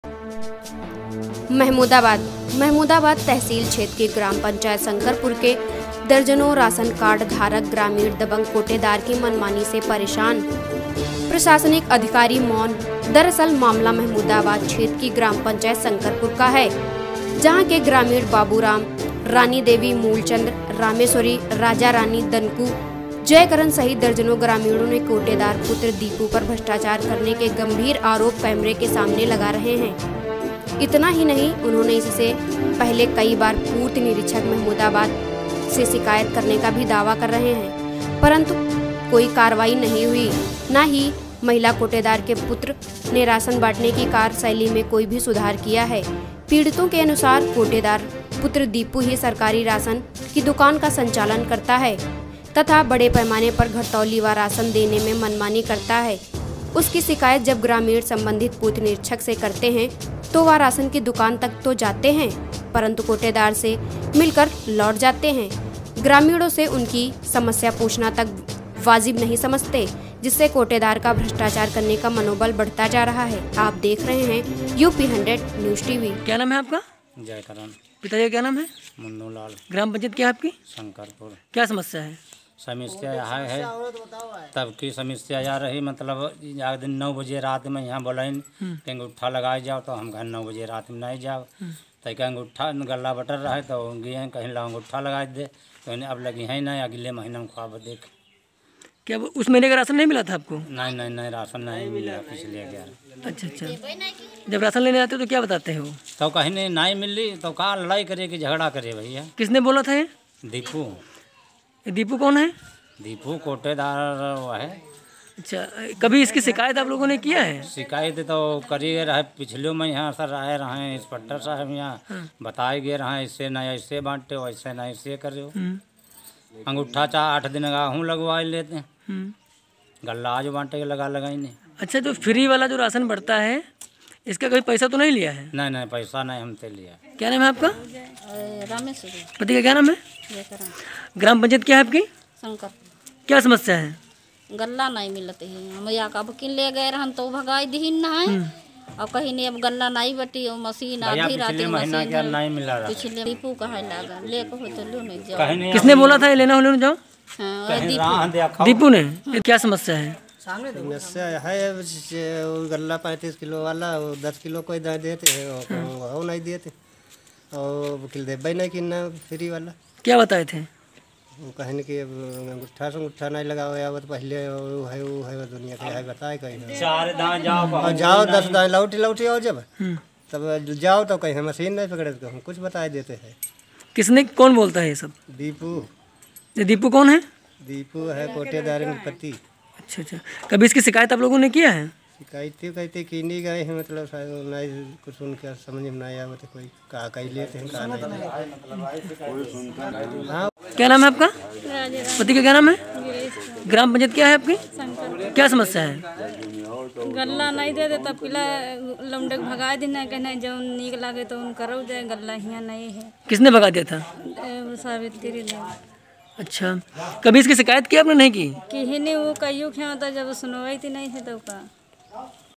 ग्राम पंचायत शंकरपुर के दर्जनों राशन कार्ड धारक ग्रामीण दबंग कोटेदार की मनमानी से परेशान
महमूदाबाद
महमूदाबाद तहसील क्षेत्र की ग्राम पंचायत शंकरपुर के दर्जनों राशन कार्ड धारक ग्रामीण दबंग कोटेदार की मनमानी से परेशान , प्रशासनिक अधिकारी मौन।।
दरअसल मामला महमूदाबाद क्षेत्र की ग्राम पंचायत शंकरपुर का है जहां के ग्रामीण बाबूराम रानी देवी मूलचंद्र रामेश्वरी राजा रानी दनकू जयकरन सहित दर्जनों ग्रामीणों ने कोटेदार पुत्र दीपू पर भ्रष्टाचार करने के गंभीर आरोप कैमरे के सामने लगा रहे हैं इतना ही नहीं इन्होंने इससे पहले कई बार पूर्ति निरीक्षक महमूदाबाद से शिकायत करने का भी दावा कर रहे हैं परंतु कार्रवाई नहीं हुई न ही महिला कोटेदार के पुत्र ने‌रासन बाटने की कार्य शैली में कोई सुधार किया। पीड़ितों के अनुसार कोटेदार पुत्र दीपू ही सरकारी राशन की दुकान का संचालन करता है तथा बड़े पैमाने पर घटतौली व रासन देने मे मनमानी करता है उसकी शिकायत जब ग्रामीण संबंधित पूर्तिनिरीक्षक से करते है तो वह रासन की दुकान तक तो जाते है परंतु कोटेदार से मिलकर लौट जाते है ग्रामीणो से उनकी समस्या पूछना तक वाजिब नही समझते जिससे कोटेदार का भ्रष्टाचार करने का मनोबल बढता जा रहा है।